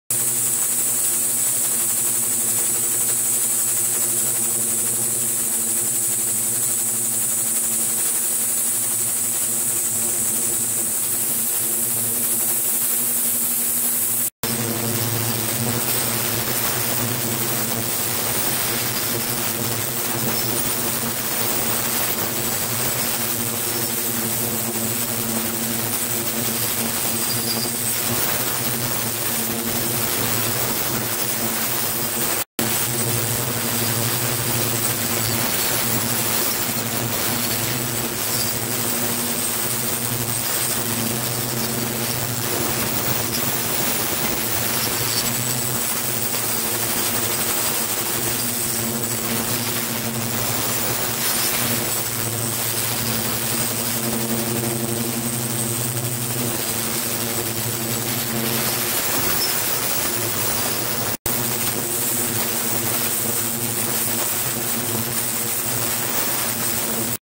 超音波（ナノテクノロジー） Ultrasonic wave (nanotechnology)
超音波（間接容器の利用）
Ultrasonic wave (Use of an indirect container)

キャビテーションと加速度の効果に関する新しい分類